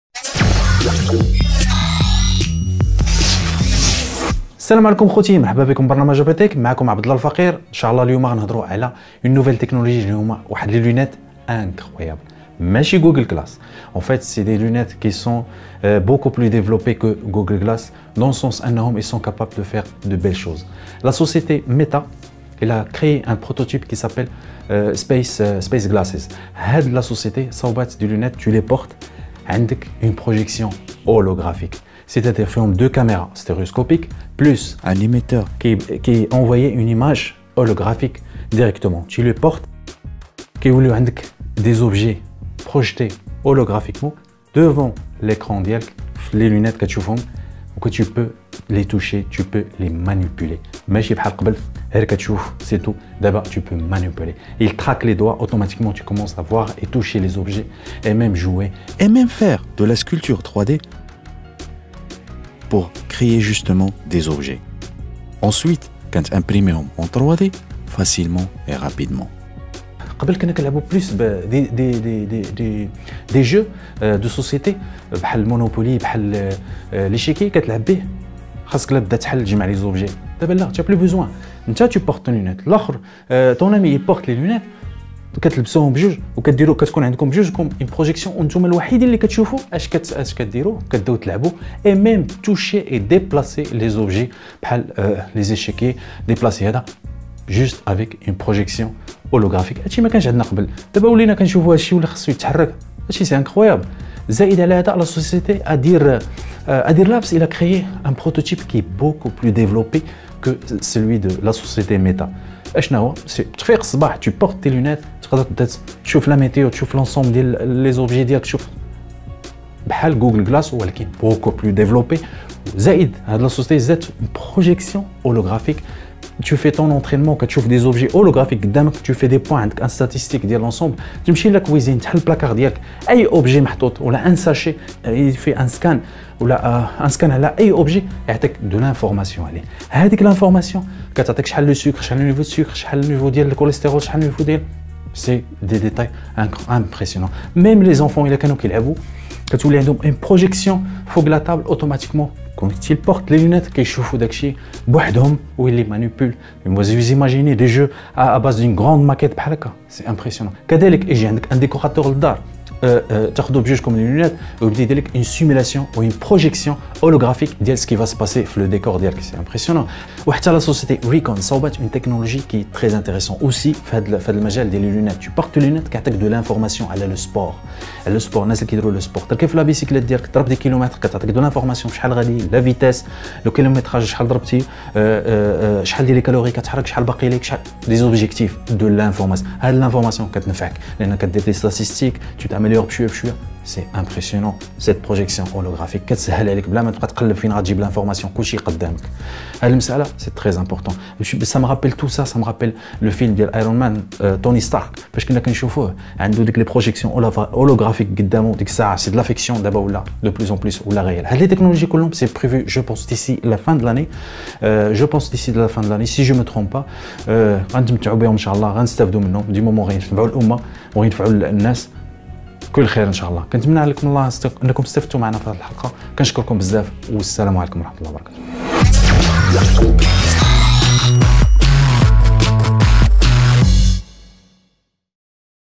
ABTech - SmartGlasses #14
Abdallah EL Fakir nous explique rapidement La technologie des Smart Glasses de la Société Meta, Atheer Labs et Recon Jet .les lunettes du futur.